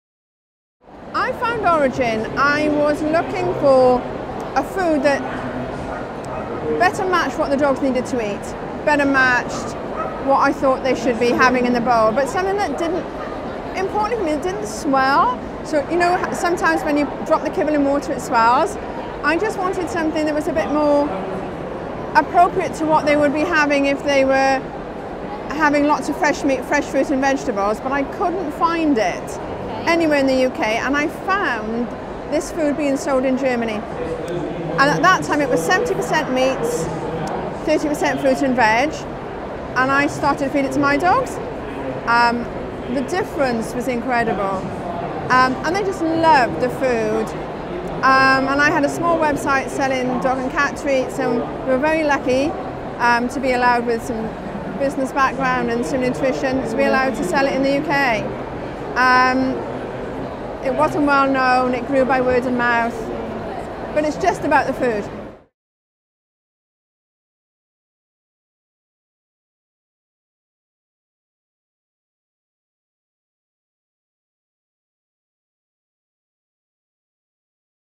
ORIJEN Pet Foods
Karen Rodger explains why she chooses to feed ORIJEN to her pets